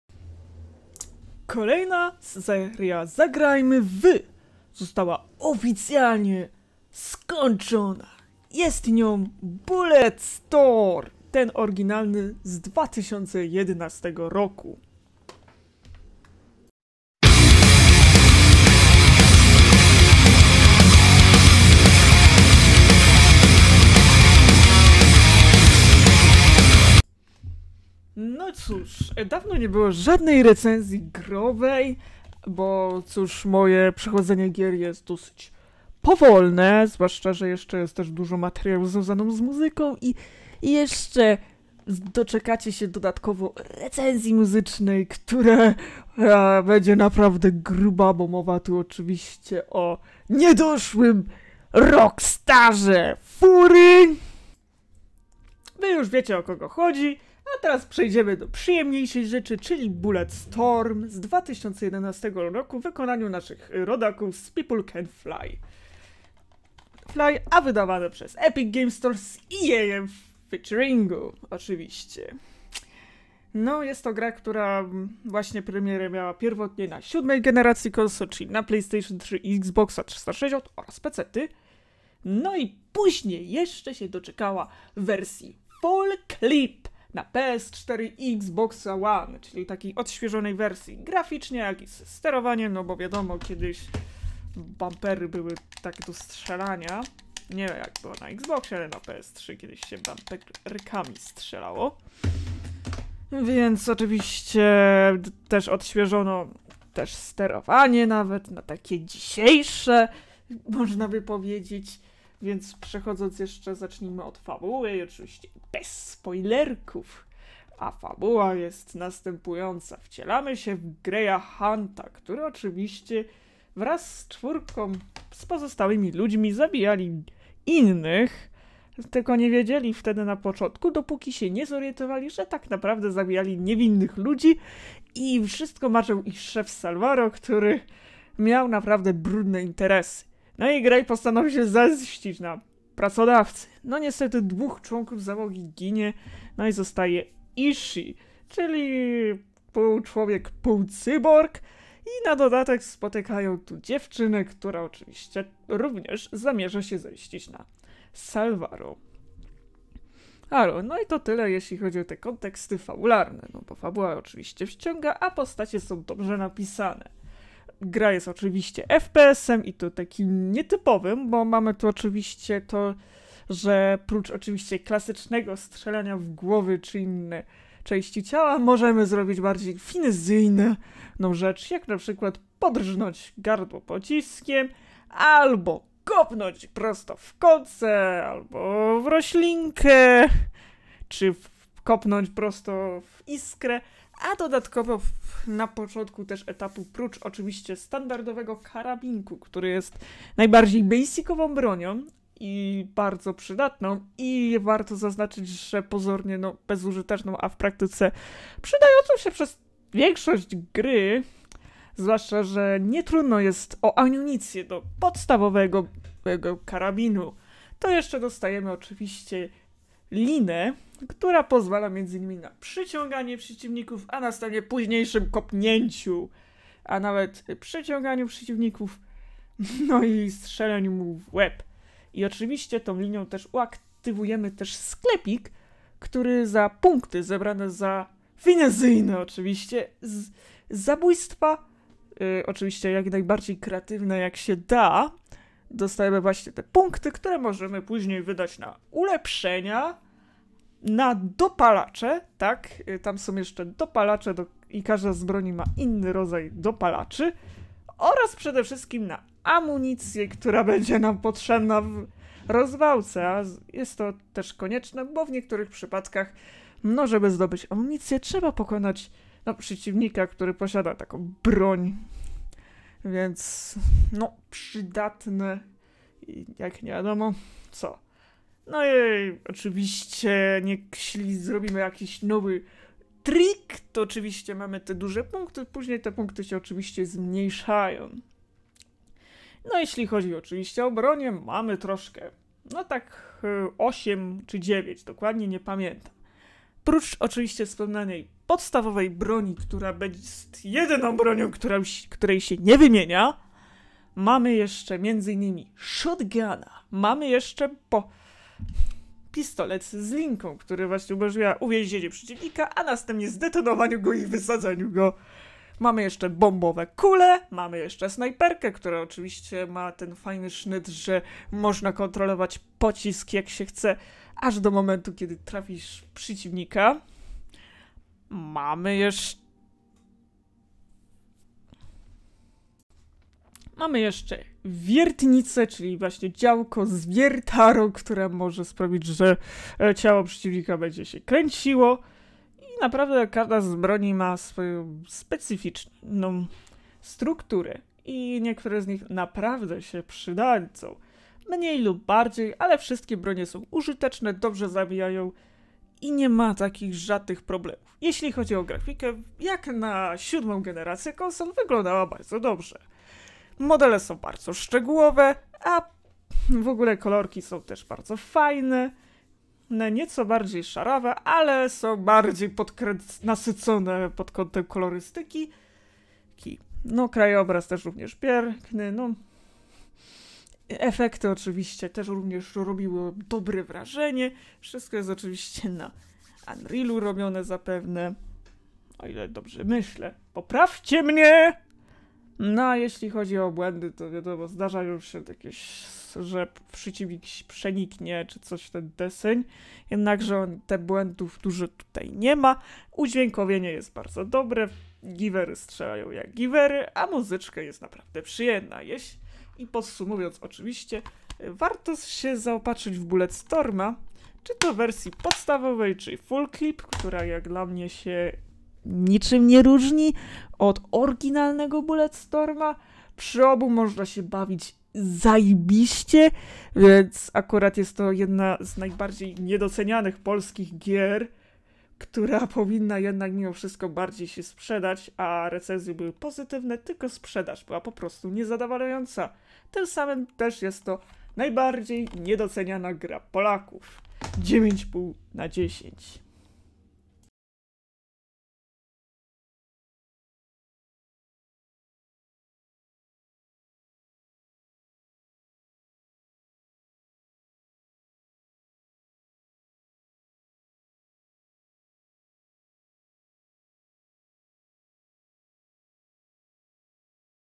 bulletstorm - recenzja
dziś będzie recenzja retro tym razem chodzi o grę bulletstorm czyli najbardziej nie doceniana polska gra

9,5/10

📷mój intagram

pozostałe rzeczy